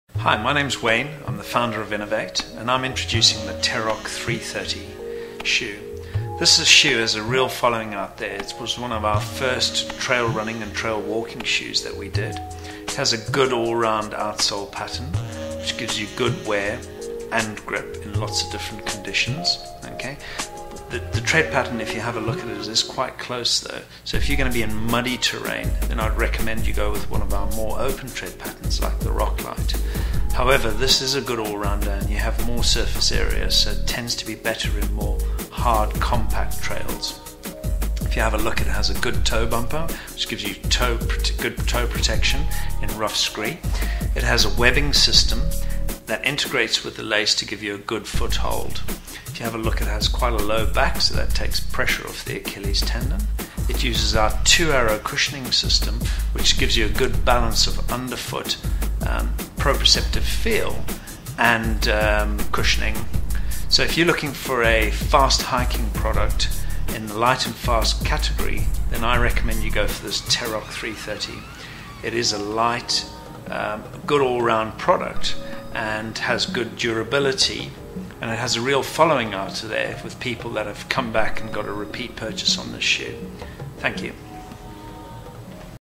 Inov-8 Terroc 330 - 365 Day Return Policy
# Running shoe built for the neutral to under-pronator who requires an enhanced cushioning and extended durability.
# The TerRoc™ 330 is a trail running and walking shoe with high levels of comfort and performance.
# Ideal for hard surface tracks and trails.
# Meta-flex®: Incorporates a groove which is anatomically aligned in front of the metatarsal heads to provide a natural fore foot flex.
# Met-cradle®: The upper webbing support is anatomically positioned to cradle the fore foot behind the metatarsal heads and provide a secure foot hold.
# Terra-shank® is a polymer shank positioned behind the metatarsal heads providing underfoot protection and extra stability primarily for additional support on longer adventures.
# Endurance rubber compound has been formulated for an optimal balance on wear and mixed terrain grip.
# Endurance last is a generous fitting last ideal for training and long distances.
# Webbing upper support is lacing attached to nylon webbing, which cradles and secures the upper behind the metatarsal heads.